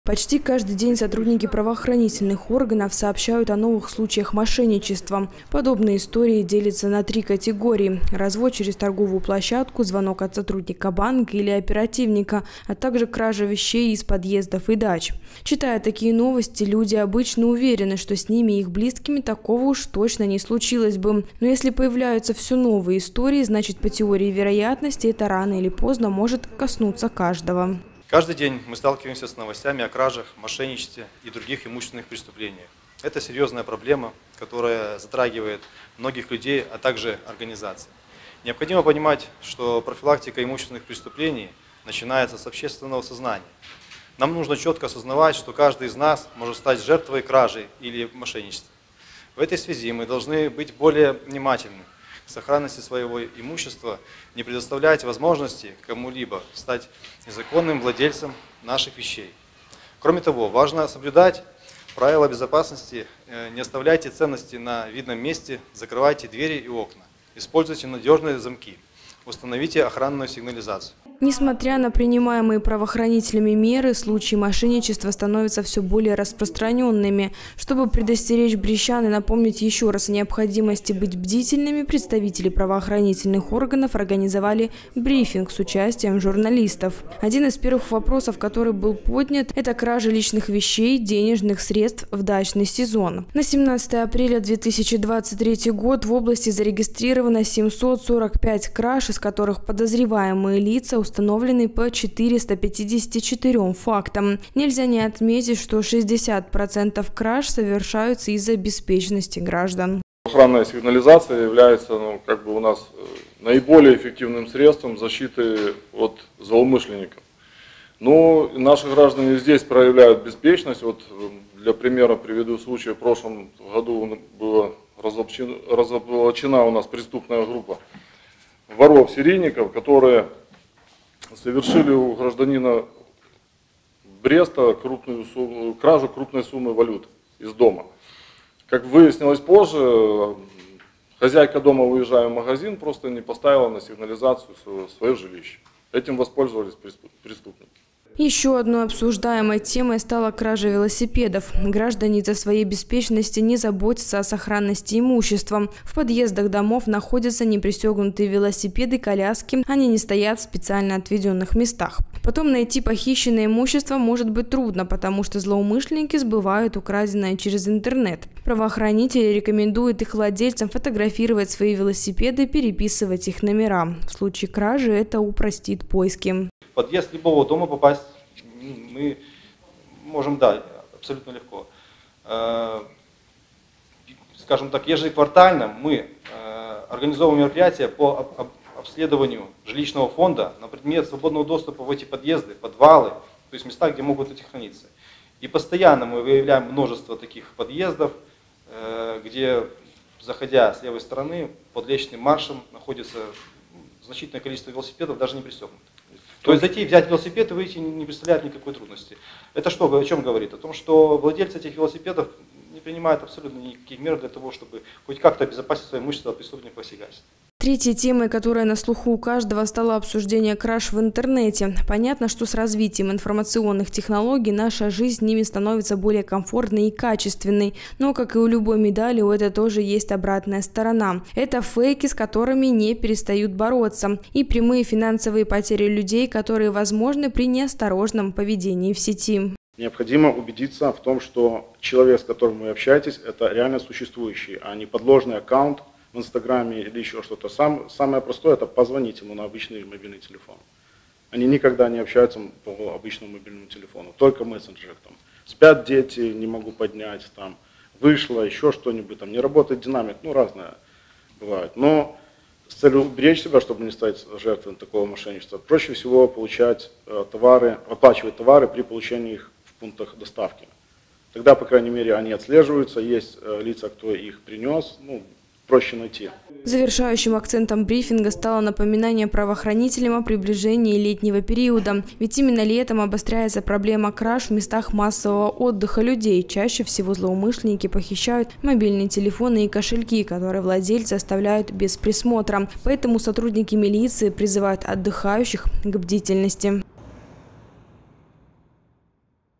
2023-04-25 г. Брест. Брифинг в УВД. Новости на Буг-ТВ.
В управлении внутренних дел состоялся брифинг. Обсуждались темы, которые касаются всех нас и каждого в отдельности. Изо дня в день правоохранители стараются максимально донести до жителей информацию о том, что такое мошенничество, на какие ухищрения идут преступники, чтобы завладеть чужими денежными средствами и как максимально защитить себя от их преступных деяний. Всего в Брестском регионе в первом квартале сотрудники внутренних дел зарегистрировали 173 мошенничества. Каждое третье из них правоохранители раскрыли. Остальные совершены в интернете, что накладывает сложности. Обсуждению этих тем и был посвящён брифинг, который состоялся в управлении внутренних дел Брестского облисполкома.